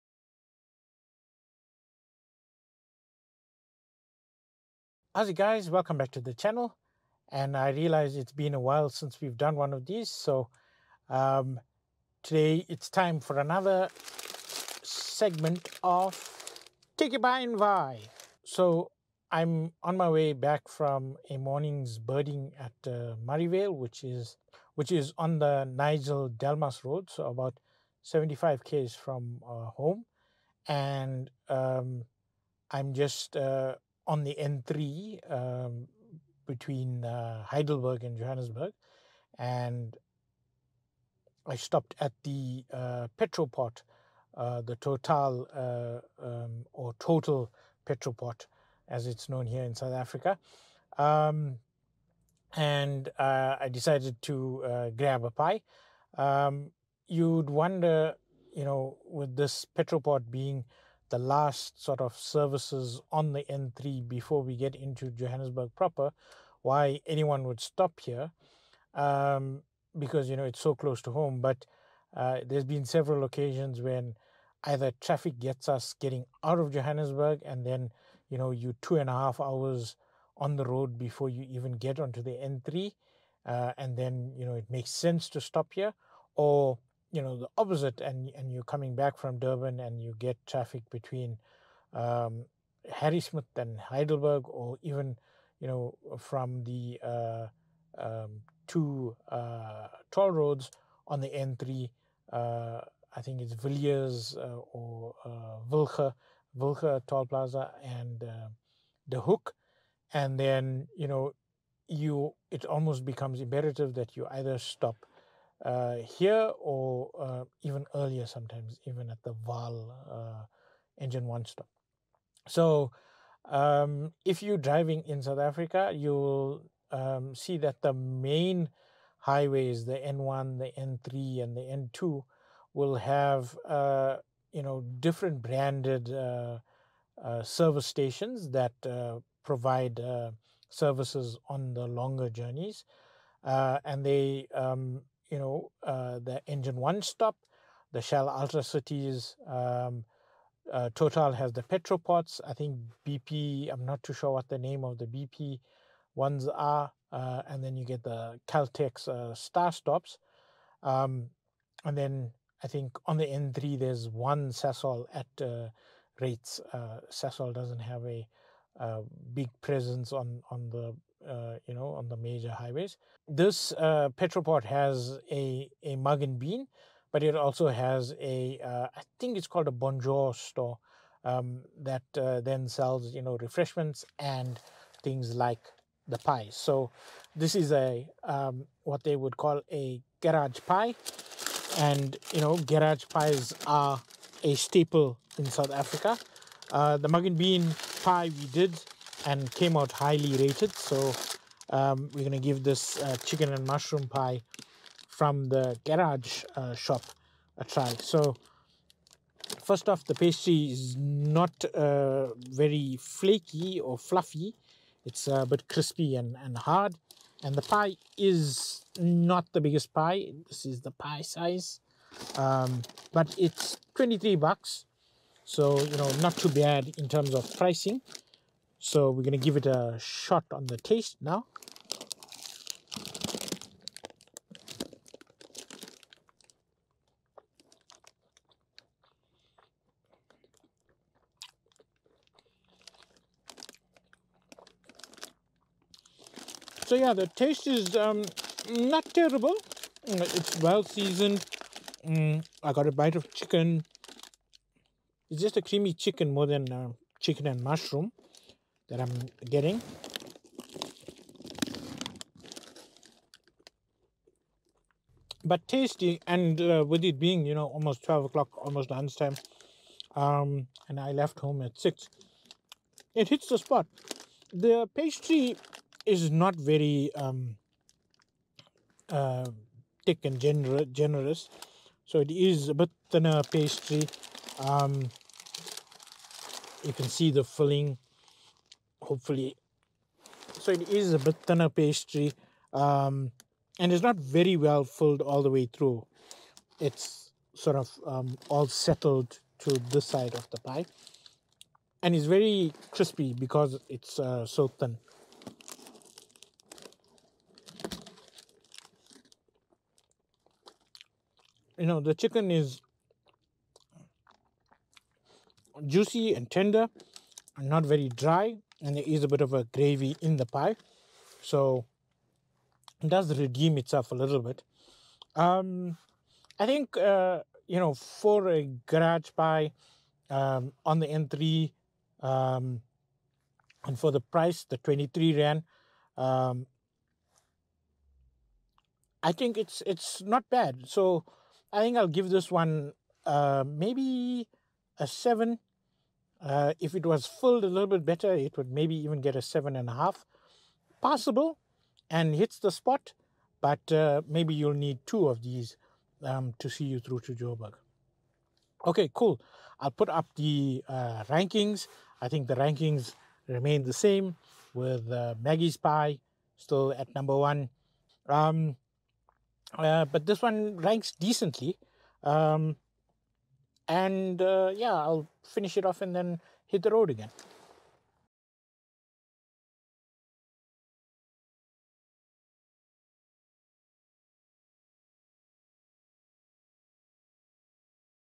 Take your pie Episode 8: A garage pie from the Bonjour Cafe in the TotalEnergies service on the N3
Hey guys! It's been a while since I have done an episode of take your pie and vaai - for this episode I review the first true garage pie on the channel, a chicken and mushroom pie from the Bonjour Cafe at the Total Energies services stop on the N3 between Johannesburg and Durban.

The N3 is one of the best serviced routes with most people opting to stop closer to Harrismith which is closer to the halfway mark for the Joburg Durban trip.  However there are several service stops before, in and after Harrismith that can also be used.  One of the first heading out of Joburg or last heading into Joburg is the TotalEnergies service stop or Petropoort between johannesburg and Heidelberg.

Price: R23 for chicken and mushroom pie
Pastry: Puff pastry but quite thin and therefore crispy
Size: Medium pie but filling seemed to have settled to one side of the pie
Taste: Well seasoned and the chicken was soft and juicy.  More a creamy chicken than chicken and mushroom
Verdict: Hit the spot being close to lunch time and only having had 2 rusks for breakfast!  A solid 7!